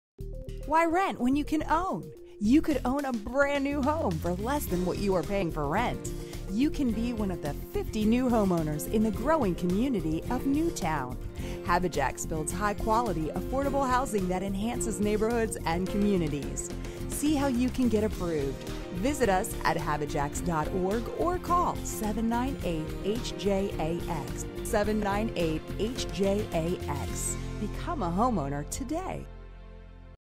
HabiJax Homeownership Commercial
HabiJax's new ad about Homeownership Opportunities in New Town